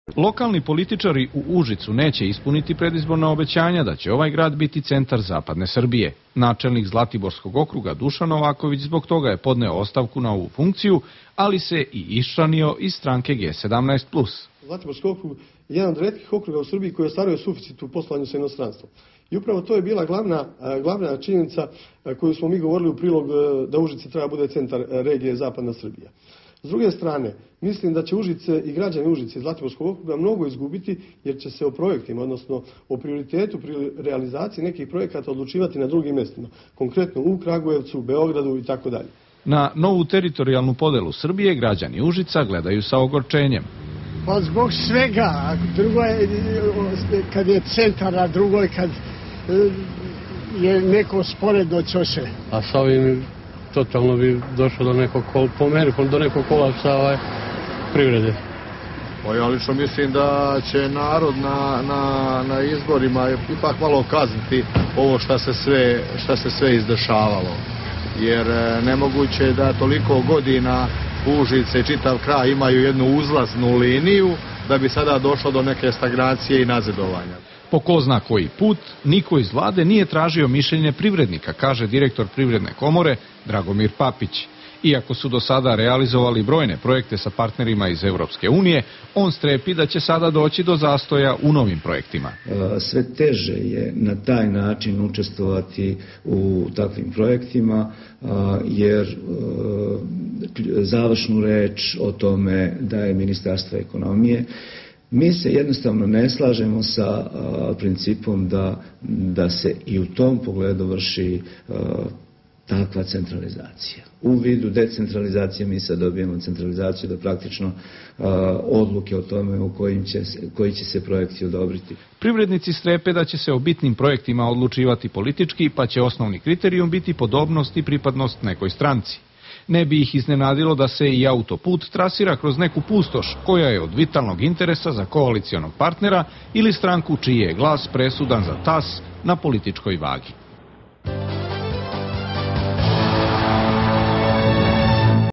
Užičani nezadovoljni regionima
Podela Srbije na pet statističkih jedinica najviše nezadovoljstva izazvala je u užičkom kraju.  To nezadovoljstvo najviše proizilazi iz negativnog iskustva preseljenja nadležnosti pojedinih javnih preduzeća iz Užica u Kraljevo, ali i podele policije na dva Sekretarijata.